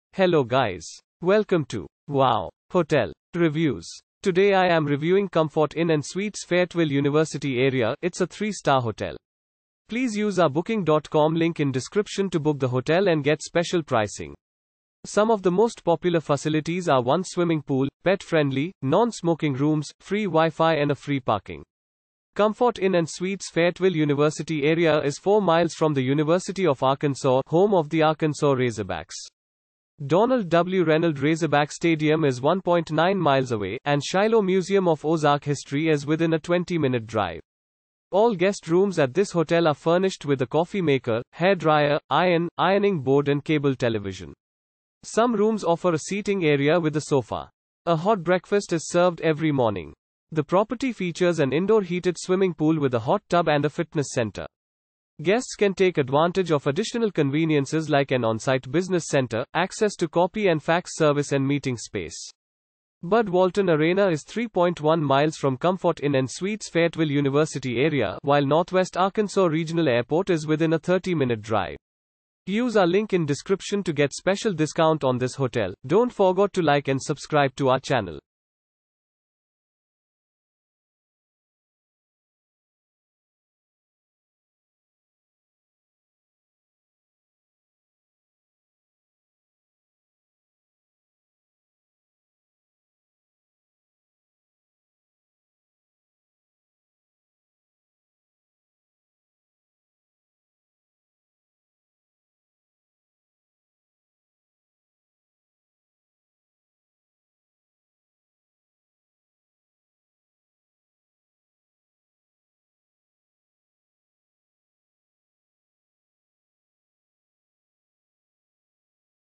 Comfort Inn & Suites Fayetteville-University Area Review - Fayetteville , United States of America
Comfort Inn & Suites Fayetteville-University Area

Comfort Inn & Suites Fayetteville-University Area is 4 mi from the University of Arkansas, home of the Arkansas Razorbacks. Donald W. Reynolds Razorback Stadium is 1.9 mi away, and Shiloh Museum of Ozark History is within a 20-minute drive.
All guest rooms at this hotel are furnished with a coffee maker, hairdryer, iron, ironing board and cable television. Some rooms offer a seating area with a sofa.
A hot breakfast is served every morning. The property features an indoor heated swimming pool with a hot tub and a fitness center. Guests can take advantage of additional conveniences like an on-site business center, access to copy and fax service and meeting space.
Bud Walton Arena is 3.1 mi from Comfort Inn & Suites Fayetteville-University Area, while Northwest Arkansas Regional Airport is within a 30-minute drive.